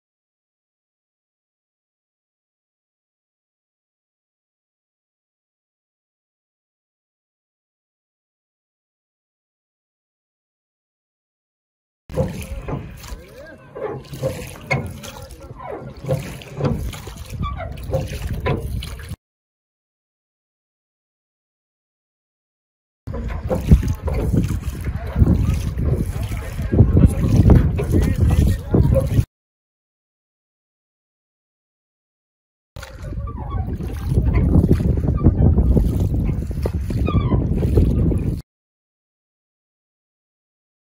DIY Rowing rig for my Grumman Canoe
Rowing rig I made by bending some exhaust pipe and clamping it on the rails of my Grumman.